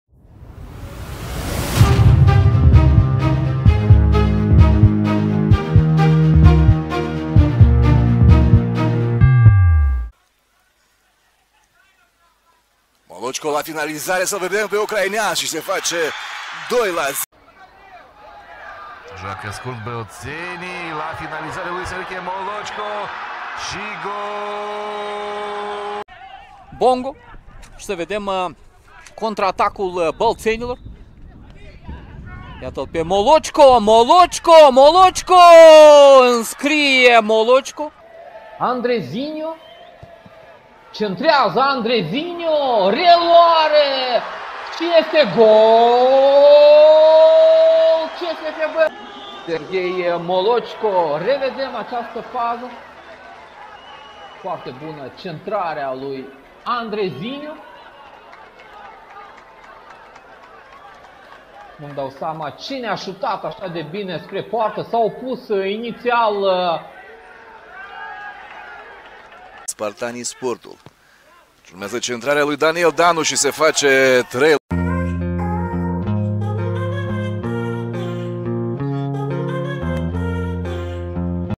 Sergiy Molochko: goals in Super Liga 2023/2024 Faza 1
GOL! — Весь молдавский футбол в одном месте. Întreg fotbalul moldovenesc într-un singur loc.

Сергей Молочко: видео всех голов, забитых в Фазе 1 Суперлиги.

Serghei Molochko: video cu toate golurile marcate în Faza 1 a Super Ligii.

Sergiy Molochko: video of all the goals scored in Faza 1 of the Super Liga.

Video source: MEDIA SPORT / Telegram (@mediasport88)